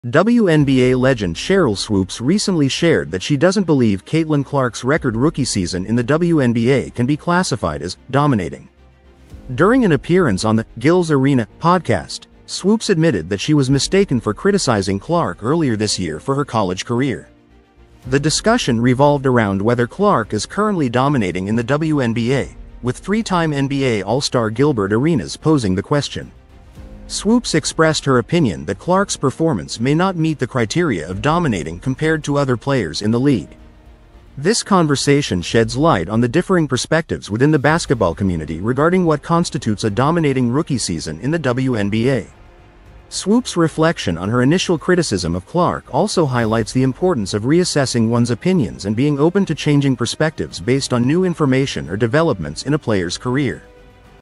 Sheryl Swoopes Doubts Caitlin Clark's Record Rookie Season is Truly 'Dominating'
WNBA legend Sheryl Swoopes recently expressed skepticism about Caitlin Clark's record-setting rookie season, questioning whether it truly qualifies as "dominating." Despite Clark's impressive achievements on the court, Swoopes suggests that the term might be used too loosely in describing the young player's impact. This discussion has sparked conversations among basketball fans and analysts, as they weigh in on whether Clark's performance lives up to the hype. Explore the debate and join the conversation about what it really means to dominate in the WNBA.